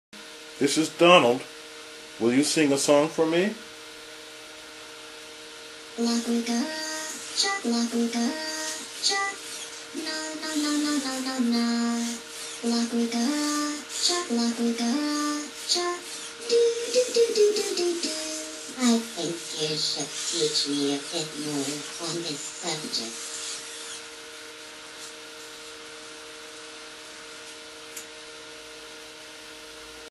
Donald Dixon Teddy Sings
Meet Alpha Teddy. Alpha Teddy was developed and created by Donald Dixon to teach children English. He also sings quite well. Alpha program was written in an artificial intelligence language called AI Creaturebot developed by Donald Dixon.